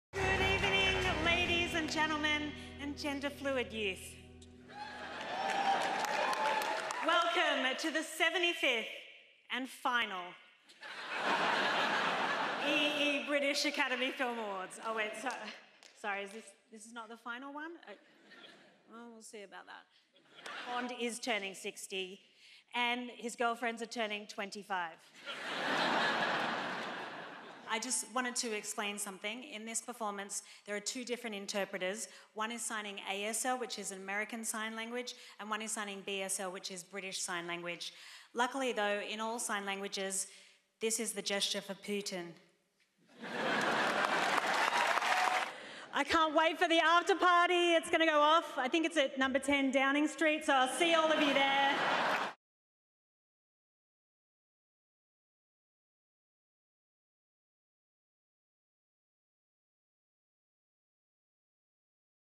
BAFTA Awards 2022: Rebel Wilsons funniest moments at London's Royal Albert Hall
Rebel Wilson hosted the 75th BAFTA Awards at London's Royal Albert Hall and threw in a fair few jokes throughout the night.